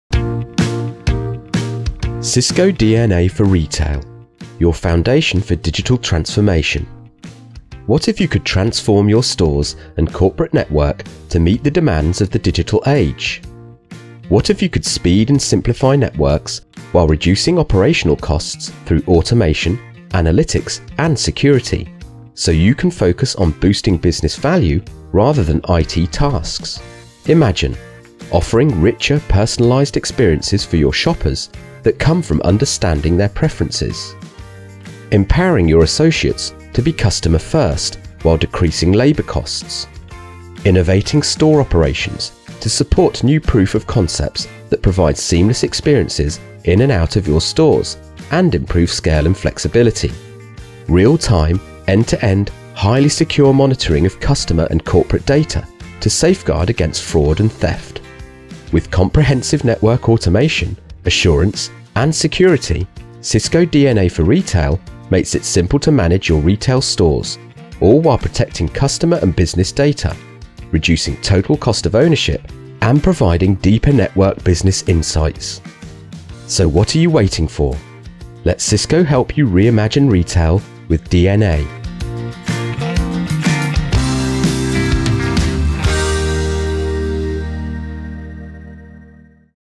Cisco Digital Network Architecture (DNA) for Retail
Cisco Digital Network Architecture (DNA) for Retail is revolutionizing how retailers design, build, and manage the business network.